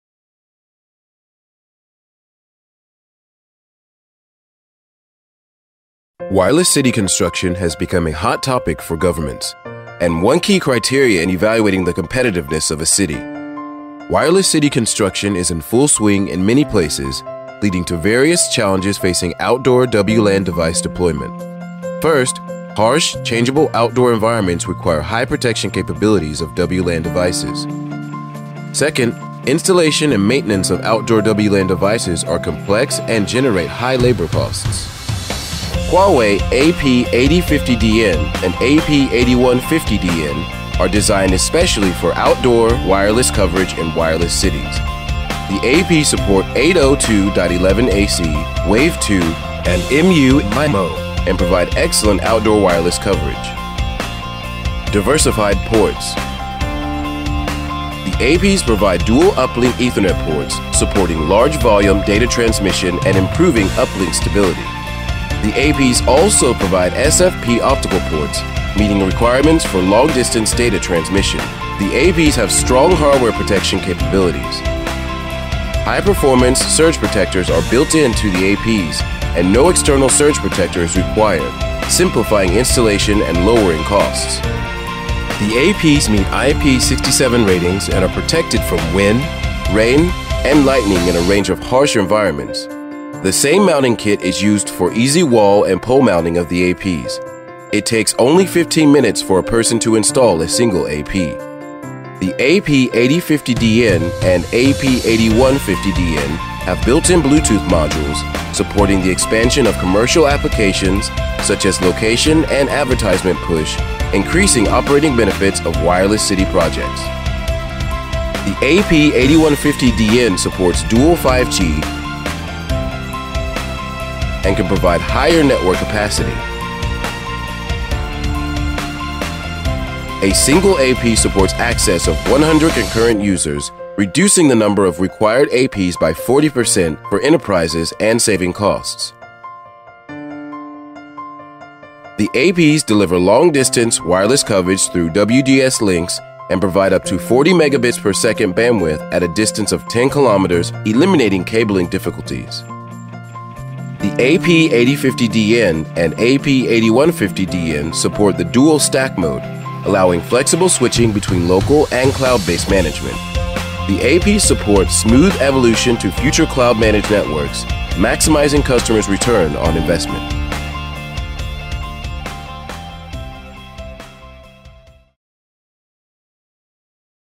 Huawei WLAN AP8050DN and AP8150DN Product Introduction
This video introduces Huawei's cost-effective and high performance 802.11ac Wave 2 AP8050DN and AP8150DN.